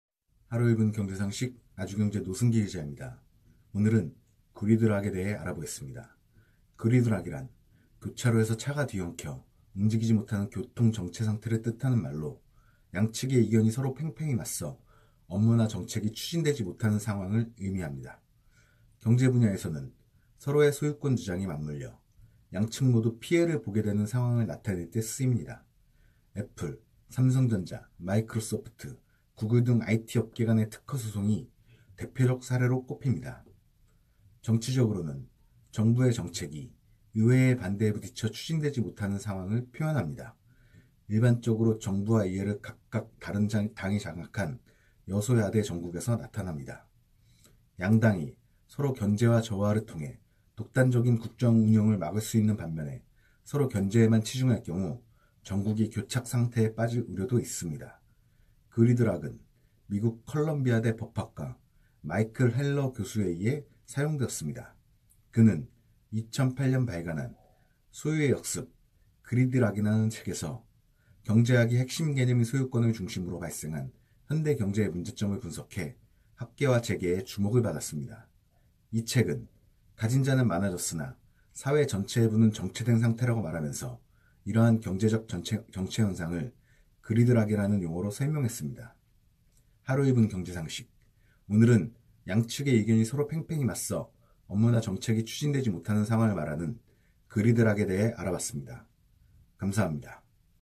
[세종24시] 하루 1분 경제상식 - '그리드락(gridlock)'
하루 1분 경제상식 아주경제 노승길 기자입니다.

오늘은 '그리드락(gridlock)'에 대해 알아보겠습니다.

그리드락이란 교차로에서 차가 뒤엉켜 움직이지 못하는 교통정체상태를 뜻하는 말로, 양측의 의견이 서로 팽팽히 맞서 업무나 정책이 추진되지 못하는 상황을 의미합니다.

경제 분야에서는 서로의 소유권 주장이 맞물려 양측 모두 피해를 보게 되는 상황을 나타낼 때 쓰입니다. 애플, 삼성전자, 마이크로소프트, 구글 등 IT 업계 간의 특허소송이 대표적 사례로 꼽힙니다.

정치적으로는 정부의 정책이 의회의 반대에 부딪혀 추진되지 못하는 상황을 표현합니다.

일반적으로 정부와 의회를 각각 다른 당이 장악한 여소야대 정국에서 나타납니다. 양당이 서로 견제와 조화를 통해 독단적인 국정운영을 막을 수 있는 반면에 서로 견제에만 치중할 경우 정국이 교착 상태에 빠질 우려도 있습니다.

그리드락은 미국 컬럼비아대 법학과 마이클 헬러 교수에 의해 사용되었습니다.

그는 2008년 발간한 '소유의 역습, 그리드락'이라는 책에서 경제학의 핵심 개념인 소유권’을 중심으로 발생한 현대 경제의 문제점을 분석해 학계와 재계의 주목을 받았습니다.

이 책은 가진 자는 많아졌으나 사회 전체의 부는 정체된 상태라고 말하면서 이러한 경제적 정체현상을 '그리드락'이라는 용어로 설명했습니다.

하루 1분 경제 상식, 오늘은 양측의 의견이 서로 팽팽히 맞서 업무나 정책이 추진되지 못하는 상황을 말하는 '그리드락'에 대해 알아봤습니다.